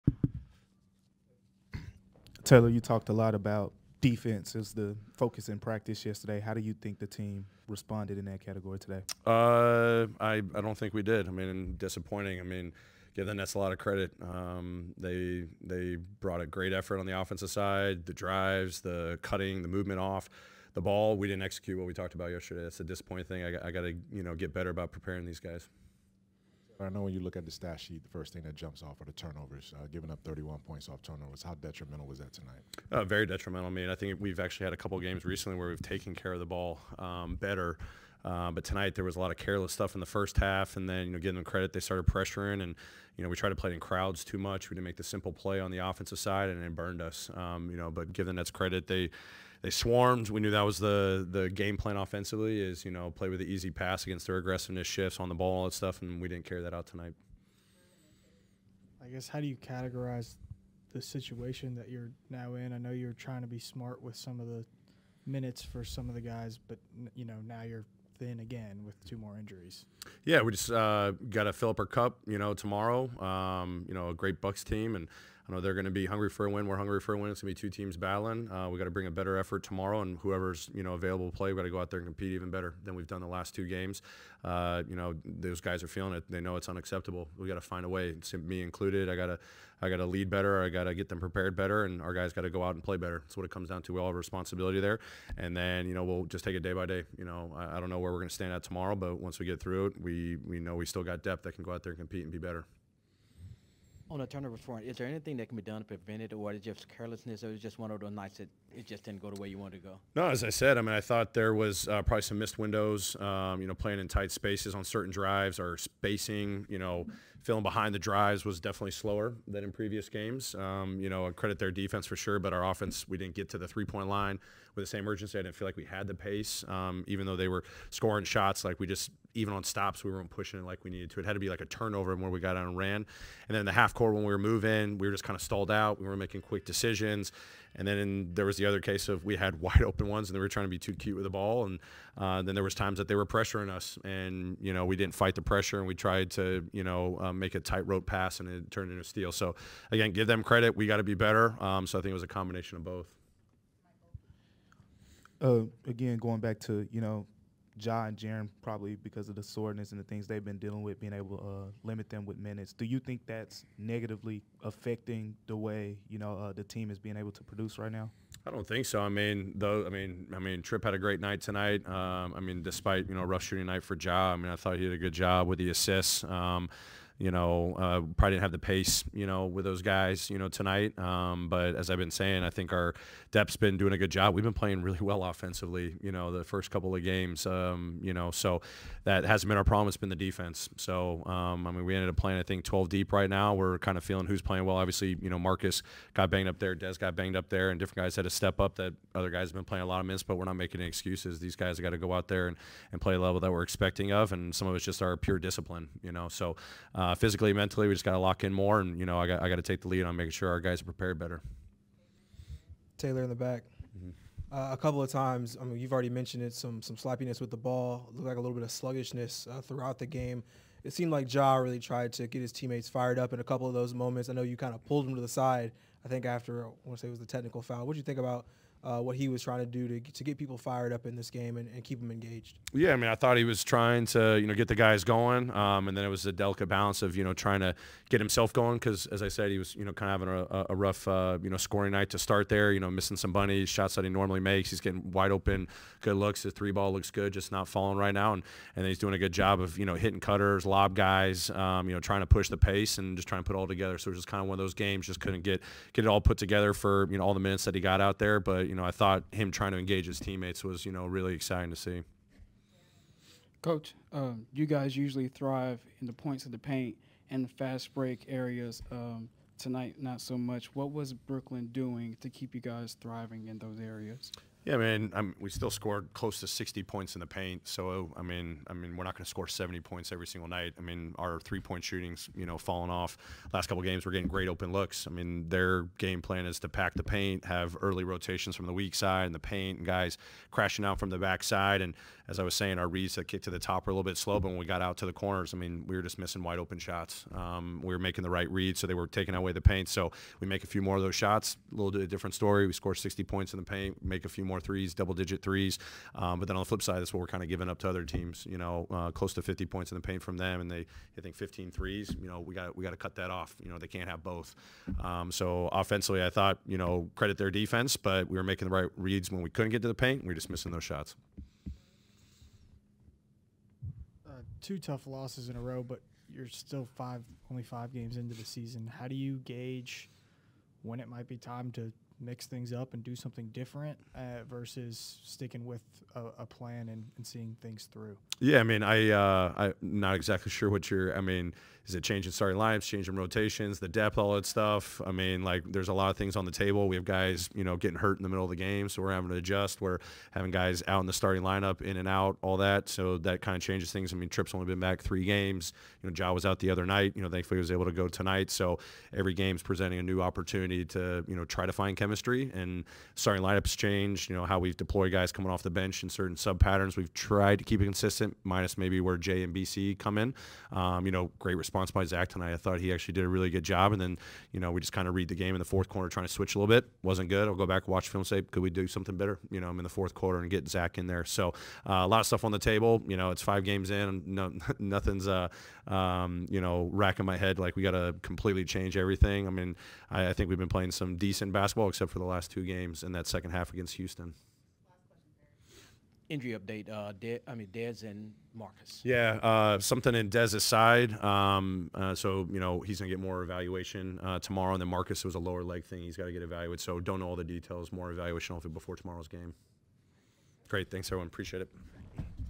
Coach Taylor Jenkins Press Conference | Grizzlies vs. Nets
Taylor Jenkins meets with the media following the Memphis Grizzlies loss to the Brooklyn Nets.